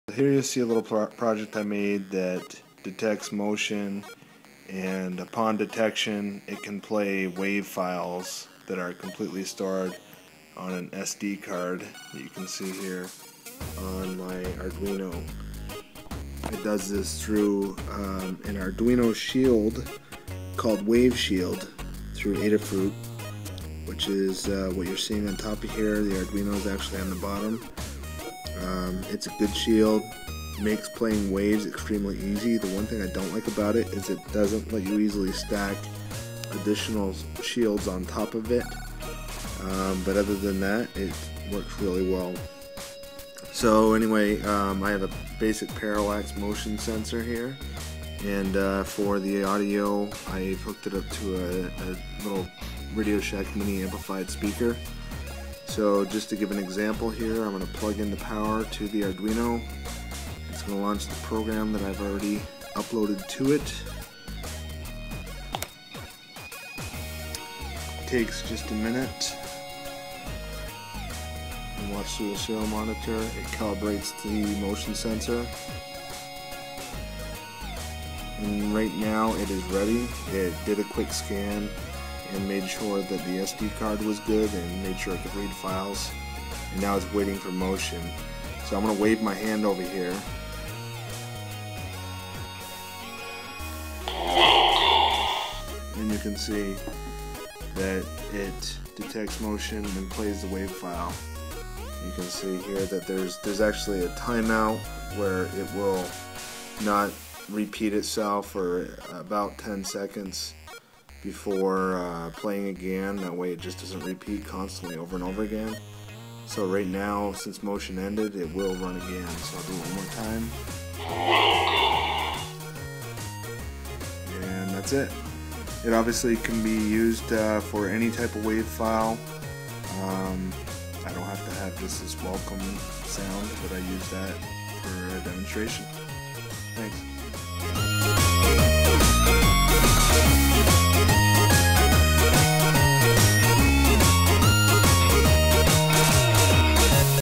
Arduino Sound Playing
Quick demonstration of using an Arduino with PIR sensor and Adafruit WavShield to play sounds when motion is detected.